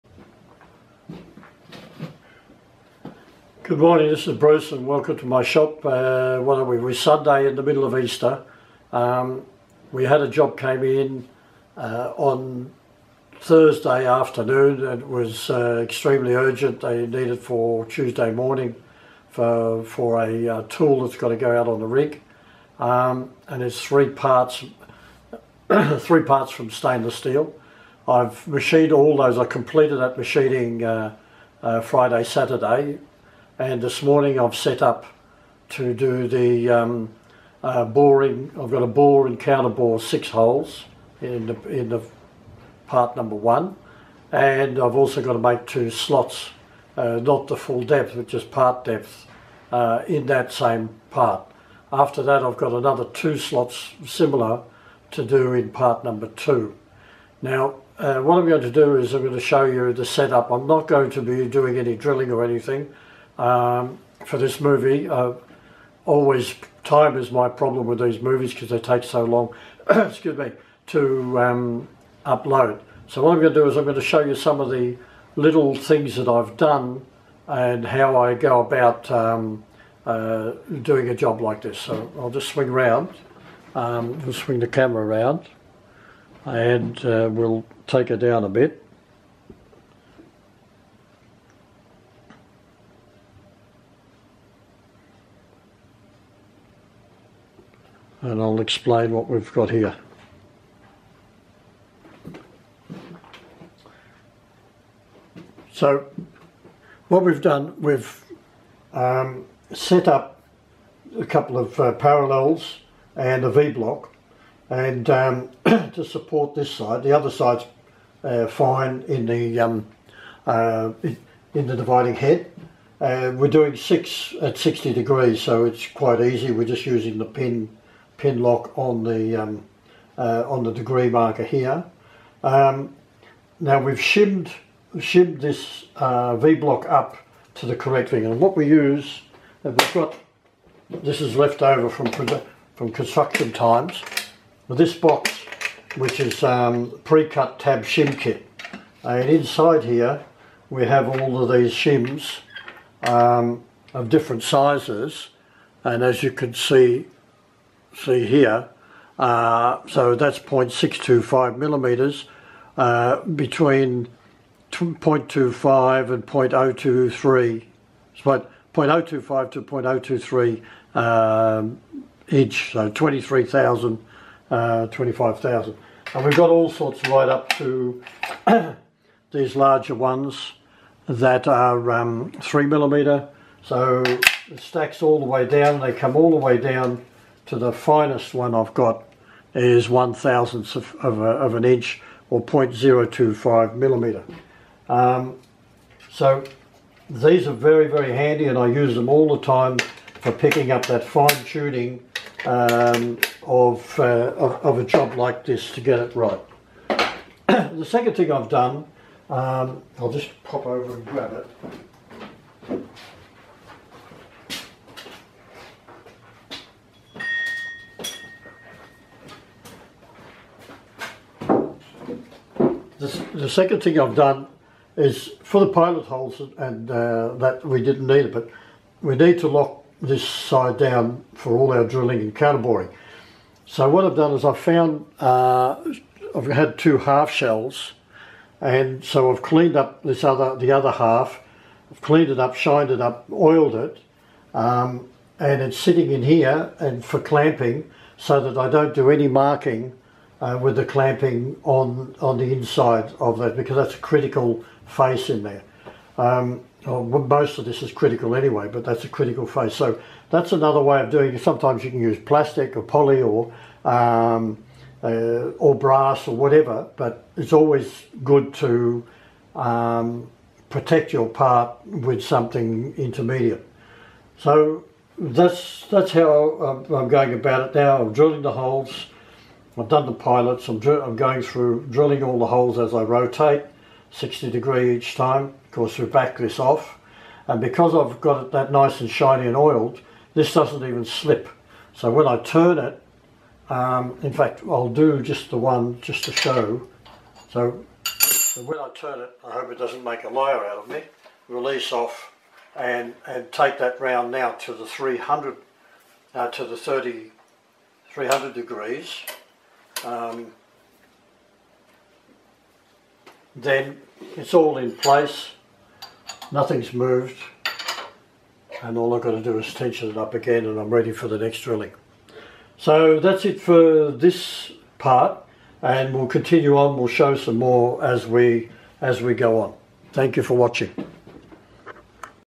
Heh Man! Easter 2016 urgent work, we must have this tool done AM Tuesday
Heh Man! Easter 2016 urgent work, we must have this tool done AM Tuesday. Special 3 part item needed for a rig. We show the setup for milling holes counterbores and slots in the tool, we also note the way we have placed a half shell inside the part for clamping. and the shims we use to provide accurate line up on the tail end support.